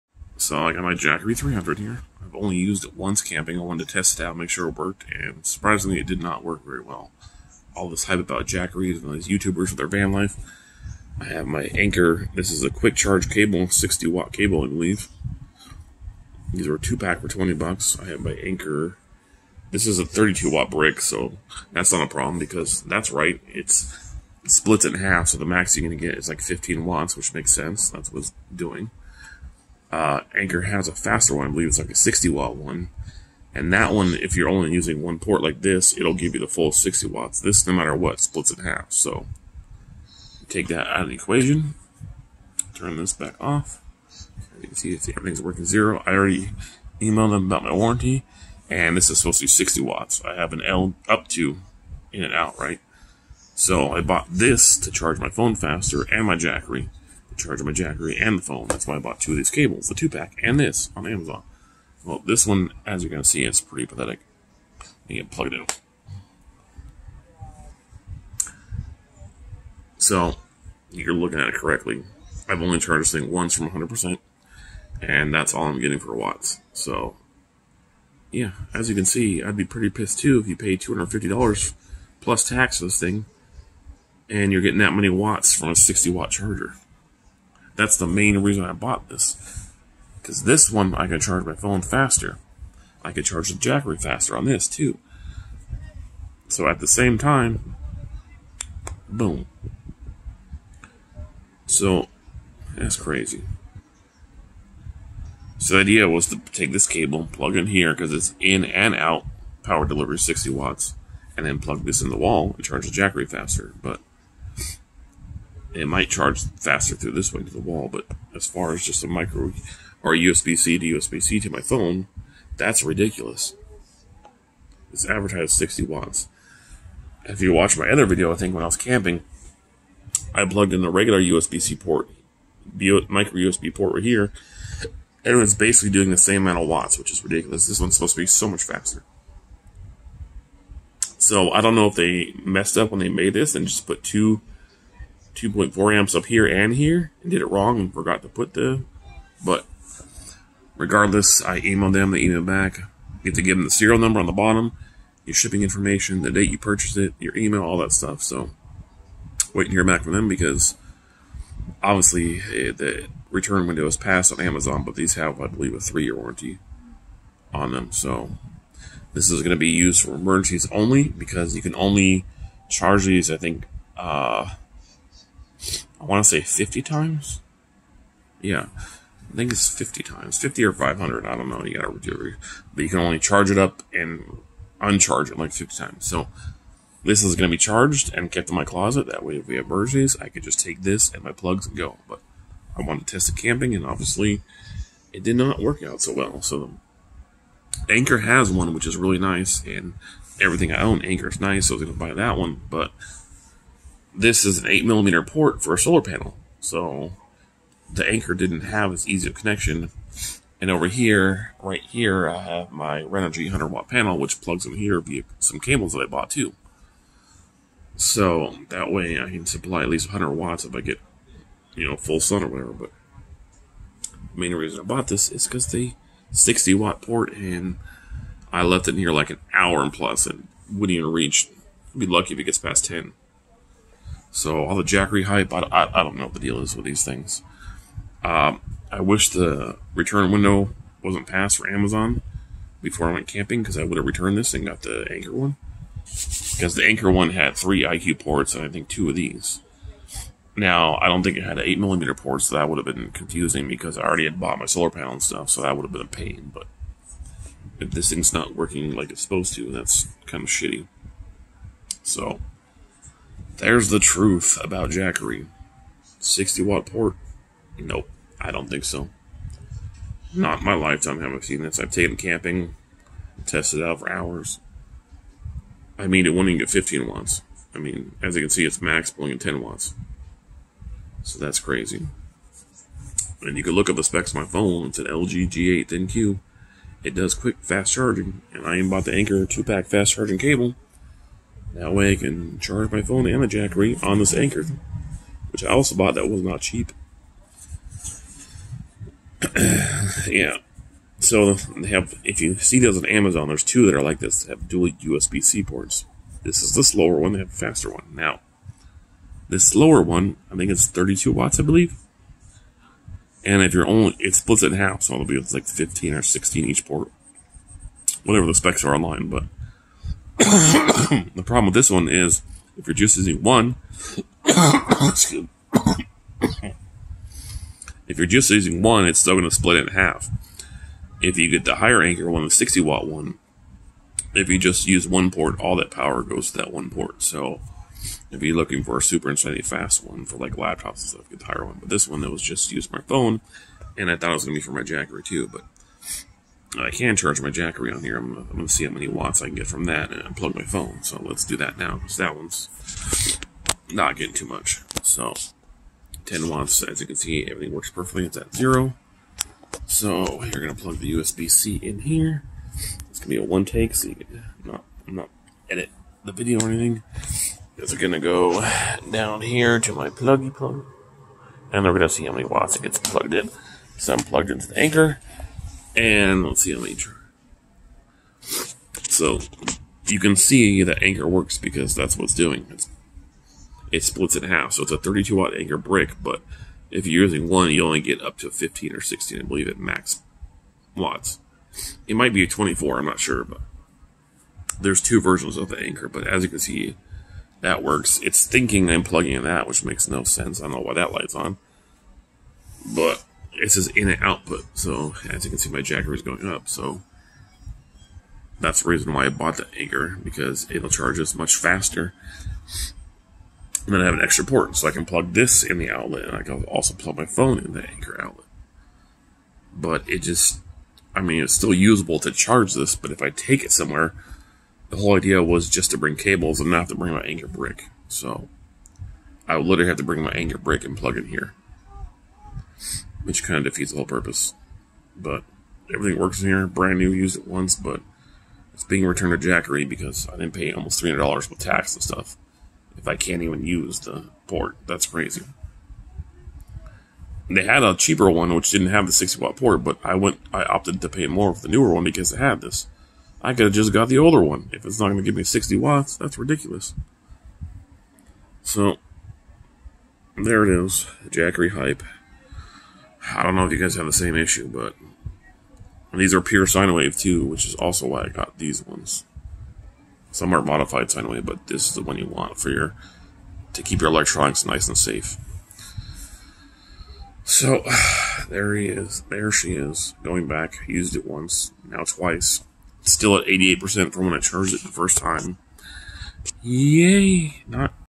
the truth about jackery 300
it's being returned advertised with a 60 watt USB c port and won't even charge above 10 watts even with my anker quick charge cable/ phone also supports quick charge so total disappointment 😫😑 I bought that new anker brick and the 2 pack of cables so i can charge my jackery and phone both faster and while camps etc use the 60 watt USB c port but that's not the case ...my anker power banks work working just as good as the jackery port kinda pissed tbh 🤬 I emailed them finally got back to me asked for info so I sent it then they email again asking I said I already sent it yesterday 🤷 like wtf and that was yesterday and nothing back today either so kinda more pissed about servicd is slow and not the most helpful either .so the truth is 💯 here hope eventually I can send it back and get a new one and it will quick charge ugh 😩